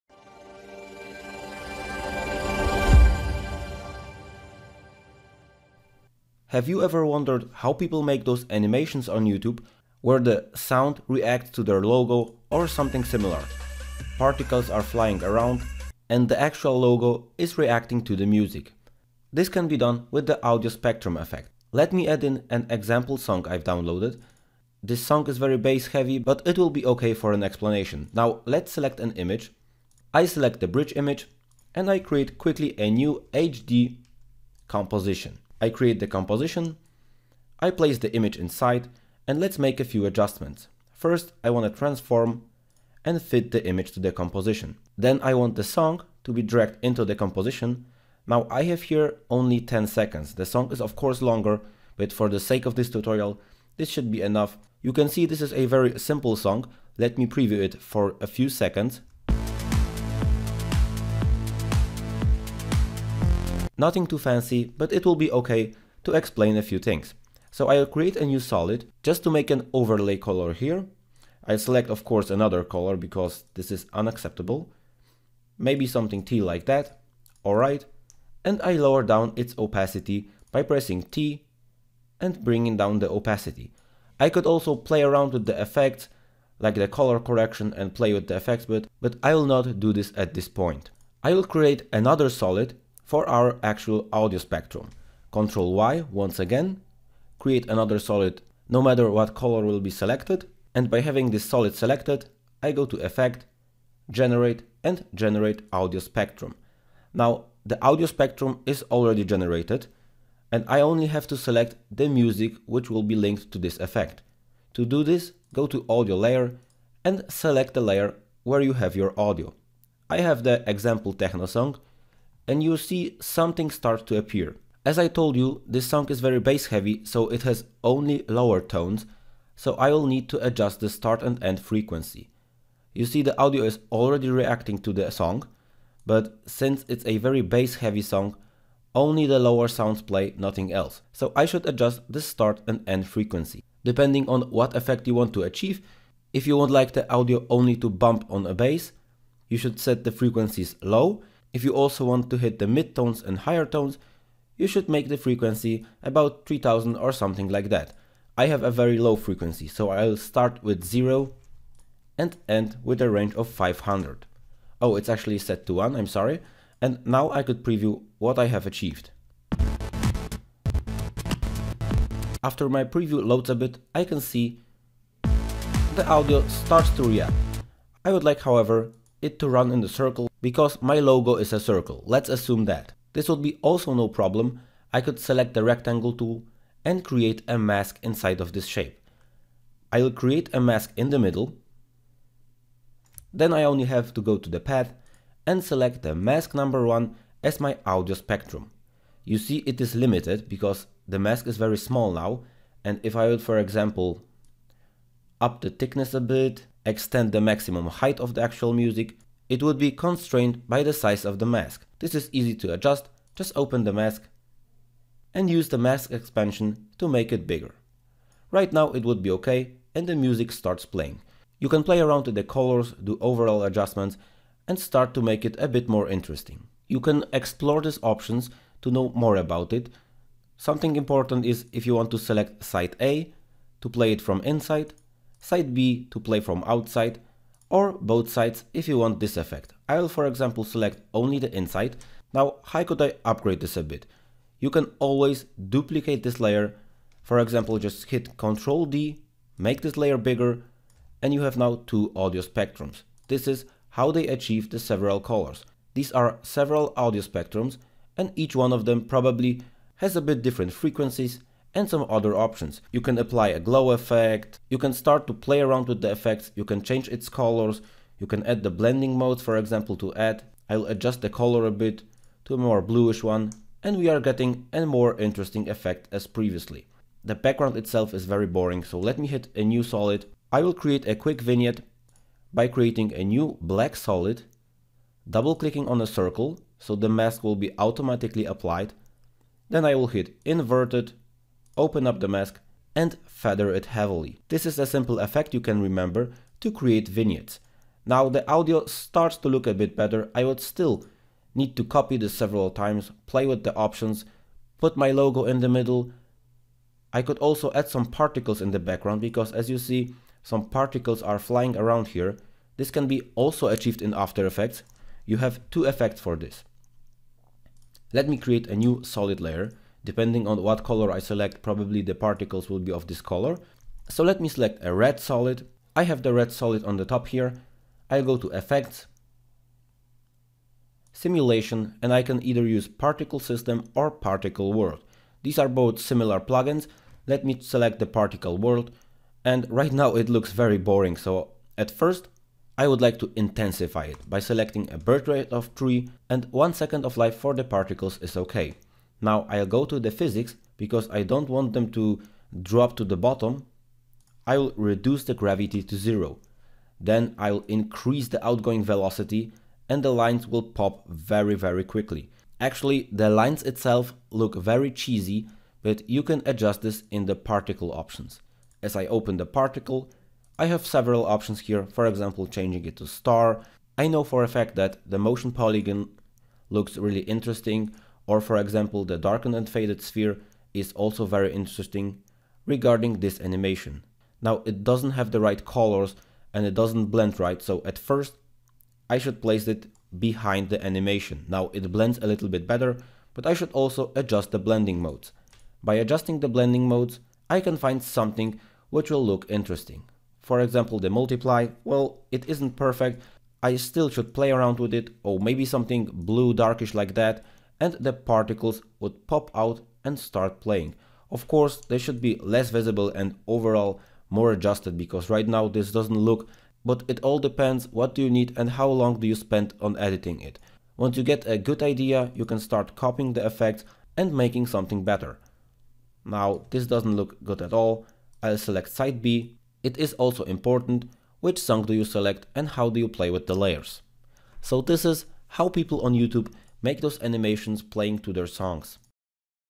After Effects Basics Complete Guide - Music Video with Audio Spectrum Tutorial
This course will teach you all neccesary & most used After Effects tools, a proper workflow, show you real-life examples of animating and various tricks in the program. Watch the above promo video for detailed explanation. This is your one-stop guidance & shortcut to professional motion graphic design.

➥ Discount links to my AE courses:
After Effects Basics Complete Guide: